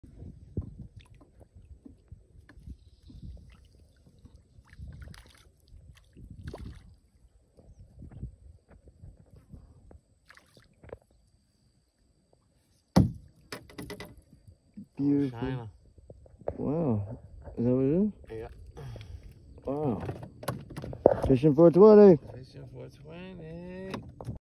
Catching a Shiner on a Fly - Crane Pond - Fishing 420
Taking the Parker River on the canoe through the salt marsh. Eventually going through Crane Pond and the Parker River out towards Plum Island. This was how tough it was! Hard day!